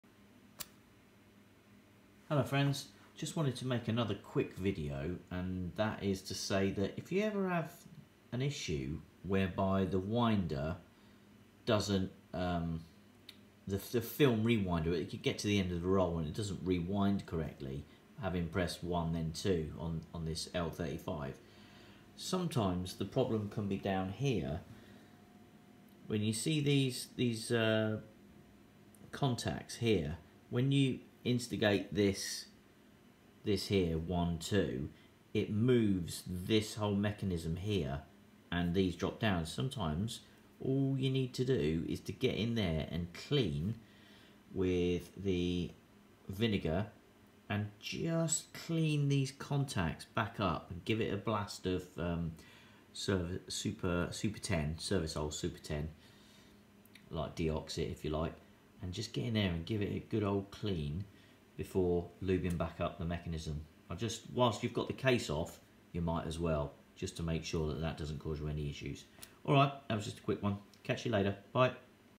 Nikon L35AF - Tips and Repairs - 4
Got an issue with your film not rewinding correctly or straight away?. its probably a build up of oxidisation upon the switch gear. This quick video highlights the area and how to fix it up.